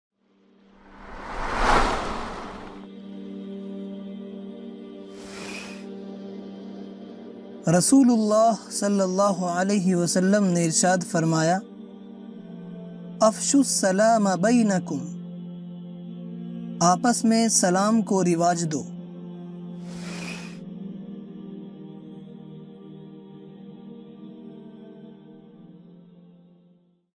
Hadith of the Day #27 | Salam ko aam karo, Islamic Salam |Urdu Hadees~Daily Reminder| IAB Studio
Recorded by Maulana Junaid Zareef
Presented by Islamic Audio Books Studio